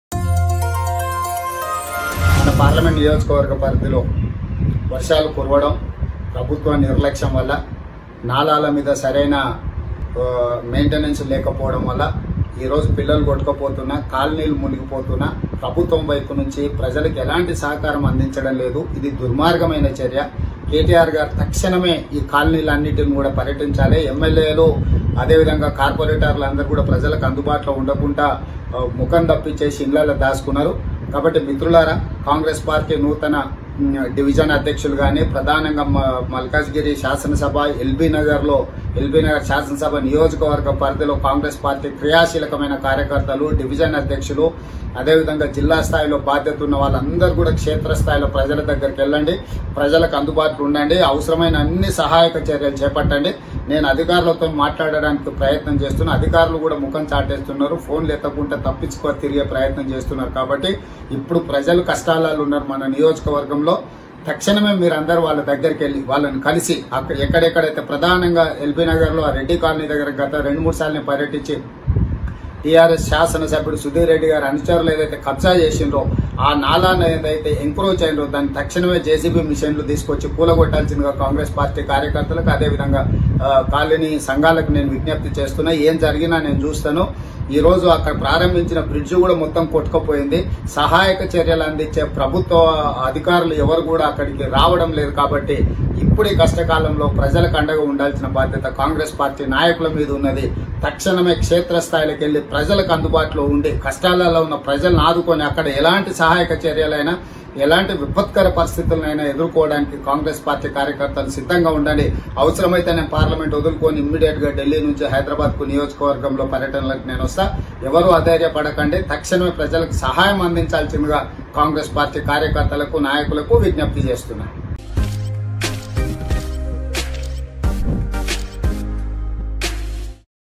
Reanth Reddy Slams KCR Govt & Orders Congress Leaders To Help Telangana People || Oneindia Telugu
Revanth reddy slams kcr government and orders to congress party leader to help telangana people who are facing problems with heavy rainfall.

పార్లమెంట్ నియోజక వర్గ పరిధిలో వర్షలు కురవడం.. ప్రభుత్వ నిర్లక్ష్యం వల్ల నాళాలపైనా సరైన మెయింటెనెన్స్ లేకపోవడం వల్ల పిల్లలు కొట్టుకుపోతున్నారని ప్రభుత్వం నుంచి ప్రజలకు ఎలాంటి సహకారం అందించడం లేదు ఇది చాలా దుర్మార్గం అని రేవంత్ రెడ్డి ఆరోపించారు.